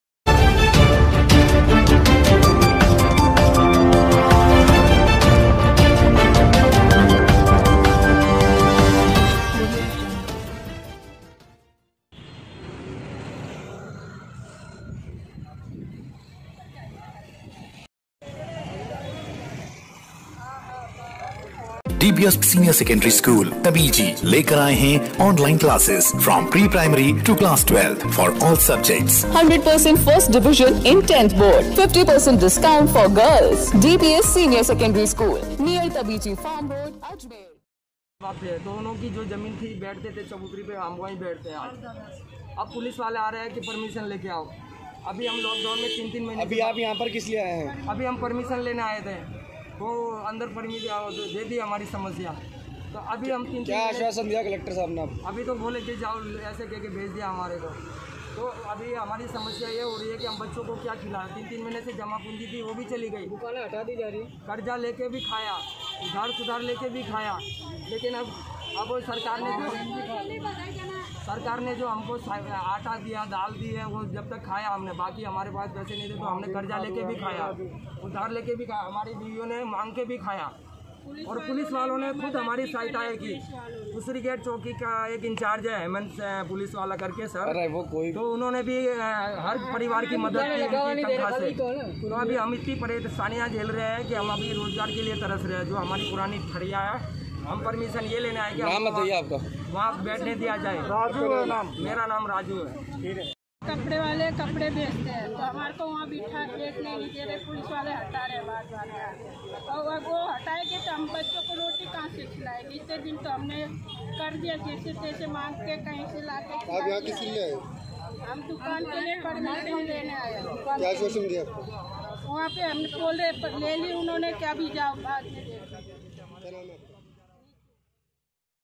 AYN NEWS : प्लाजा सिनेमा के बाहर पुराने कपड़े व्यापारियों ने ठेले लगाने की मांगी अनुमति
प्लाजा सिनेमा के बाहर पुराने कपड़े व्यापारियों ने ठेले लगाने की अनुमति हेतु कलेक्टर से मांगी परमिशन, लोक डाउन की मार झेल रहे हैं व्यापारी